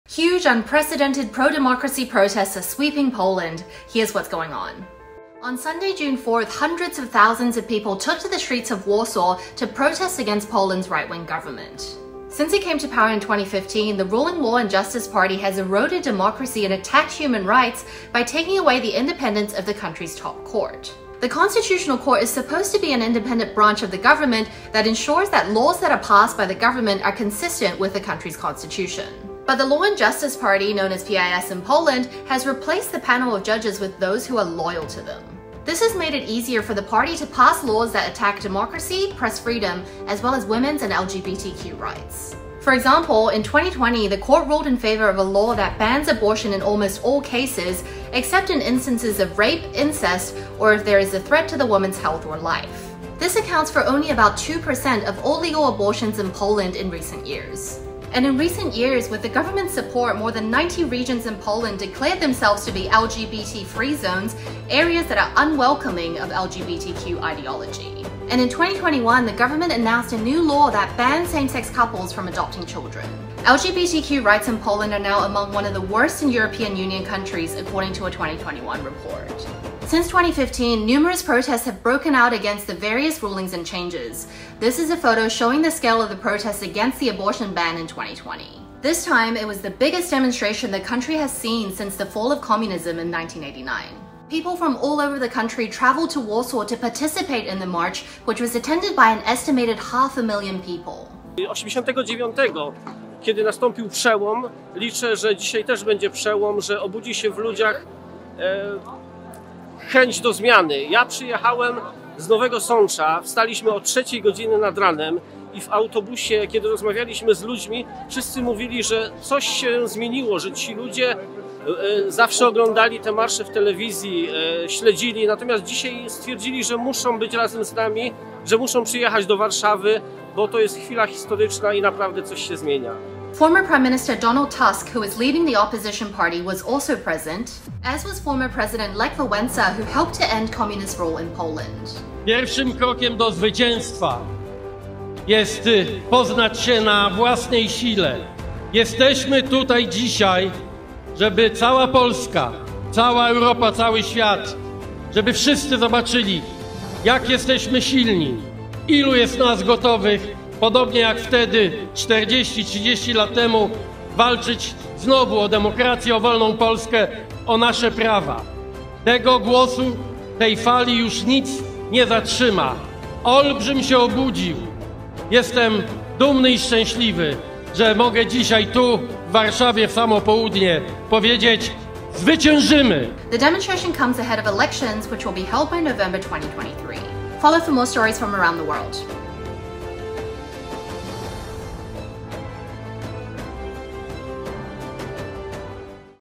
Why Are People In Poland Holding Huge, Unprecedented Anti-Government Protests?
An estimated half a million people have held a huge unprecedented pro-democracy protest in Poland. Here’s what you need to know.

On Sunday June 4, hundreds of thousands of people took to the streets of Warsaw to protest against Poland’s right-wing government.

Since it came to power in 2015, the ruling Law and Justice party has eroded democracy and attacked human rights by taking away the independence of the country’s top court.

The constitutional court is supposed to be an independent branch of the government that ensures laws passed by the government are consistent with the country’s constitution.

But the Law and Justice party, known as PiS in Poland, has replaced the panel of judges with those who are loyal to them.

This has made it easier for the party to pass laws that attack democracy, press freedom, as well as women’s and LGBTQ rights.

For example, in 2020, the court ruled in favor of a law that bans abortions in almost all cases, except in instances of rape, incest or if there is a threat to the woman’s health or life.

This accounts for only about 2% of all legal abortions in Poland in recent years.

And in recent years, with the government’s support, more than 90 regions in Poland declared themselves to be “LGBT-free” zones – areas that are unwelcoming of “LGBTQ ideology”.

In 2021, the government announced a new law that banned same-sex couples from adopting children.

LGBTQ rights in Poland are now among one of the worst in European Union countries, according to a 2021 report.

Since 2015, numerous protests have broken out against the various rulings and changes.

This time, it was the biggest demonstration the country has seen since the fall of communism in 1989.

People from all around the country traveled to Warsaw to participate in the march, which was attended by an estimated half a million people.

“In 1989 there was a breakthrough (first free elections in Poland) I hope that today there will also be a breakthrough, that people will be willing to change,” 51-year-old IT specialist Jacek Gwozdz, who attended the march, told Reuters. “I came from Nowy Sacz, we got up at 3 a.m. and when we talked to people on the bus, everyone said that something had changed. These people always watched these marches on TV, followed them. Today, however, they decided that they must be with us, that they must come to Warsaw because this is a historic moment and something is really changing.”

Former prime minister Donald Tusk, who is leading the opposition party, was also present, as well former president Lech Wałęsa who helped to end communist rule in Poland.

“The first step to victory is to recognise our strength, we’re here today so that Poland, Europe and the world see how strong we are, how many of us are ready, just like back then 30, 40 years ago, to fight again for a democratic and free Poland, for our rights,” Tusk said at the demonstration. “This voice will not be stopped, that giant has woken up, I’m proud that I can be here and say ‘we will win!’”

The demonstration comes ahead of elections which will be held by November this year.